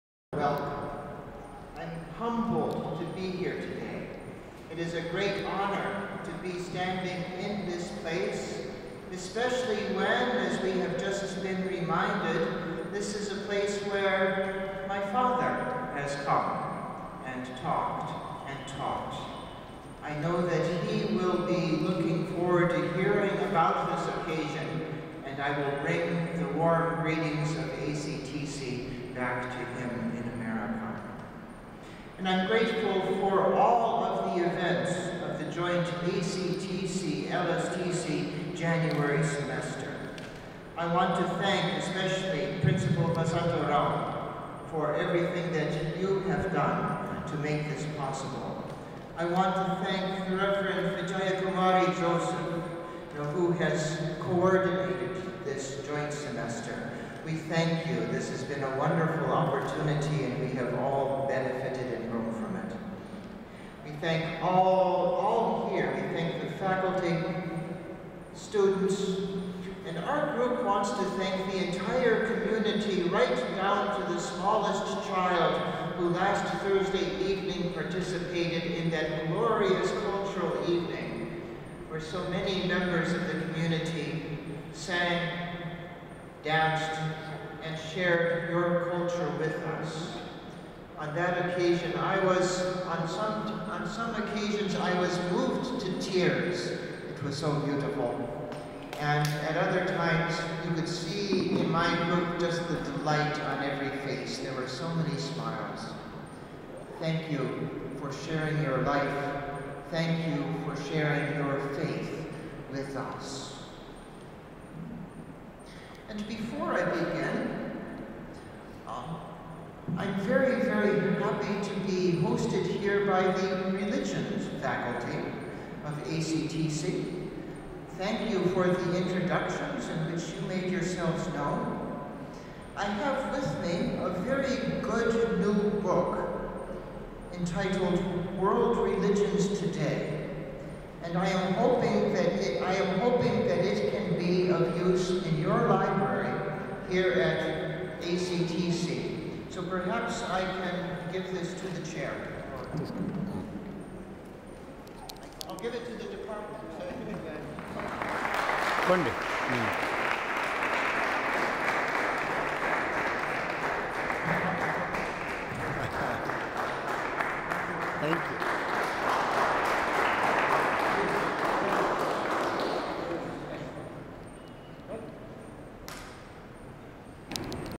ACTC Hyderabad LSTC Joint J Sem Introductory words by Dr Mark Swanson 21.01.2013
Dr Mark Swanson's introductory words on the occasion of ACTC - LSTC join January Semester at ACTC Hyderabad India and thanks Rev Dr. Chilkuri Vasantha Rao Principal ACTC and Rev. S.Vijaya Kumari Joseph, Coordinator, ACTC for organizing the JSem at ACTC.